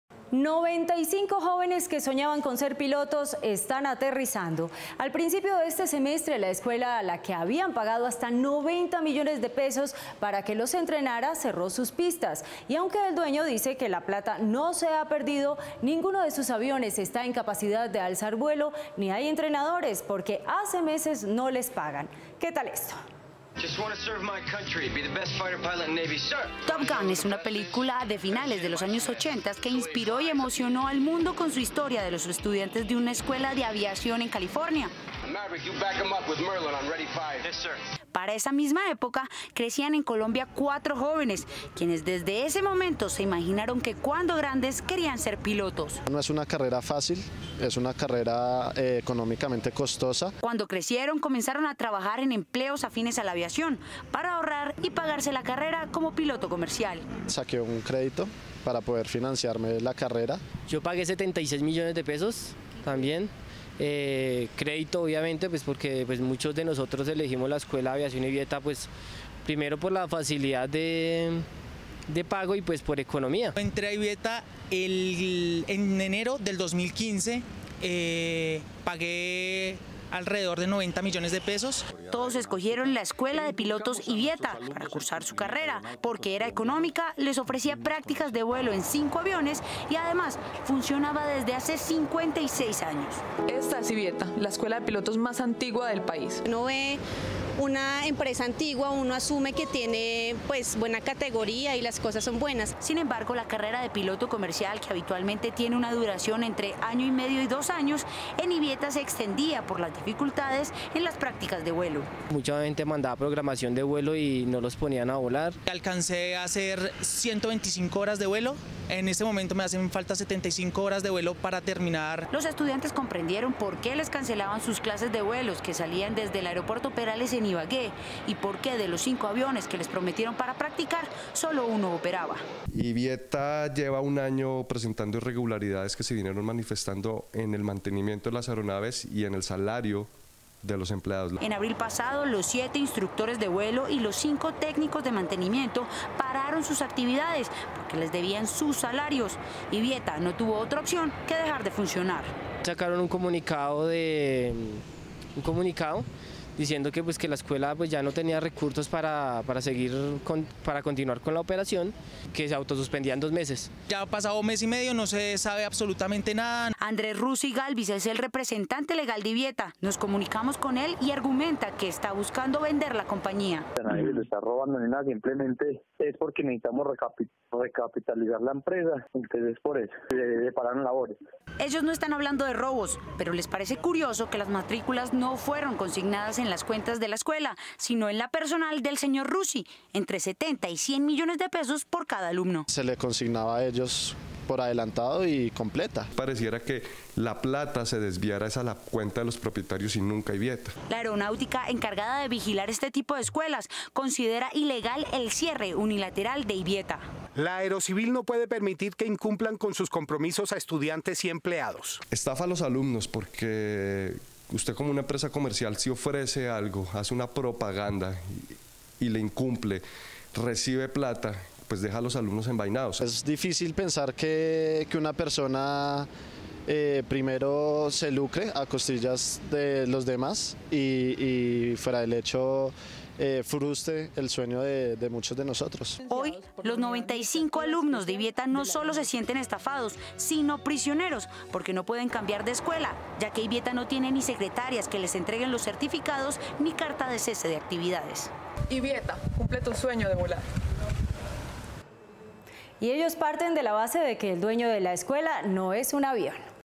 Escuela de aviación cobró matrículas de 90 millones, se declaró en quiebra y cerró sus puertas
95 jóvenes que soñaban con ser pilotos están aterrizando. Al principio de este semestre la escuela a la que le habían pagado hasta 90 millones de pesos para que los entrenara cerró sus pistas y aunque el dueño dice que la plata no se ha perdido ninguno de sus aviones está en capacidad de alzar vuelo, ni hay entrenadores porque hace meses no les pagan.